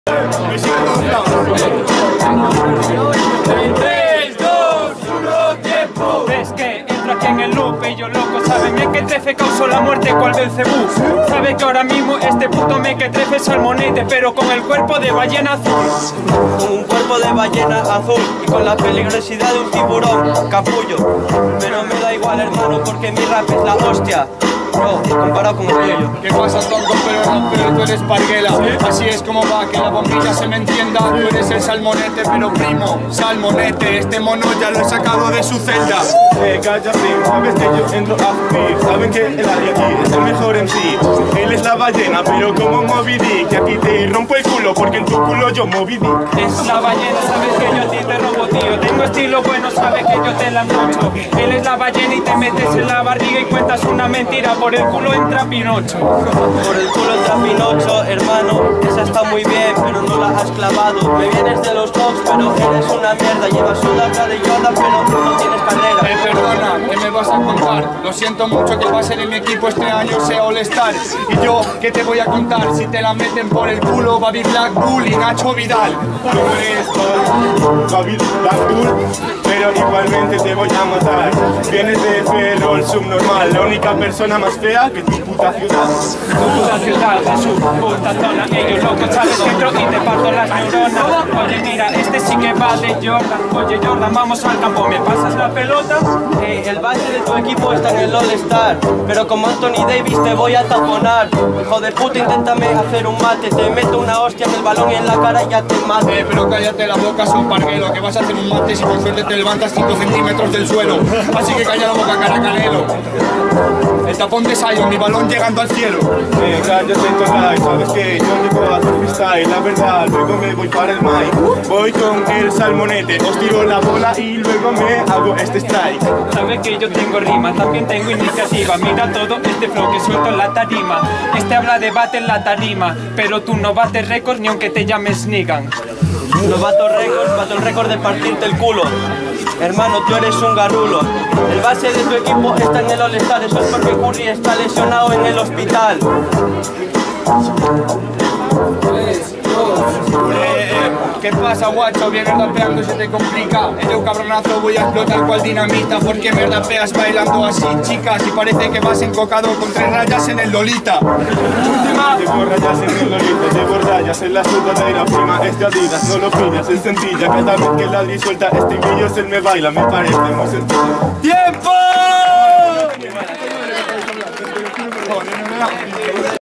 Toni y Miguez vs salmonete y Adri (filtros)
Filtros de la sexta edición de las LP battles

En esta ronda las puntuaciones de cada MC fueron individuales y al acabar pasaron los 32 mejores por eso no hay votacion del jurado.